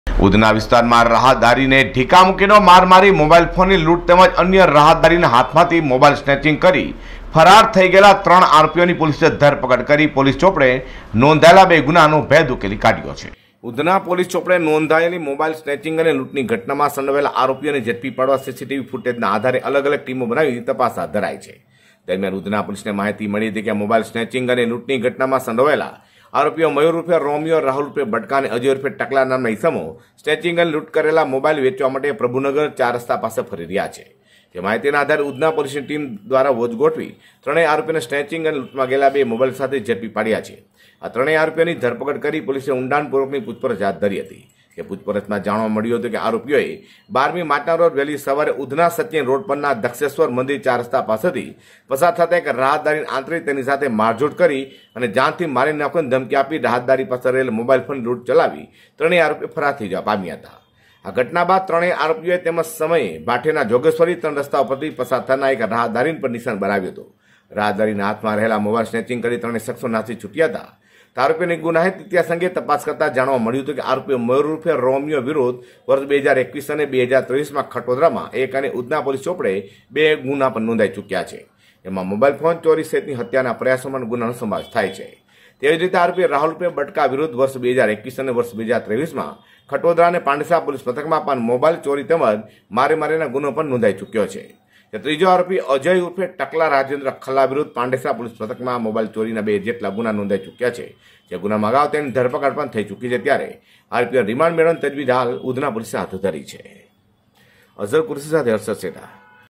જુઓ આ ત્રણ રીઢાઓ મયુર, રાહુલ અને અજય કરતા હતા મોબાઈલ લુંટ
એકલ દોકલ વ્યક્તિઓને લુંટી લેનાર ટોળકીના ત્રણને ઉધના પોલીસે ઝડપ્યા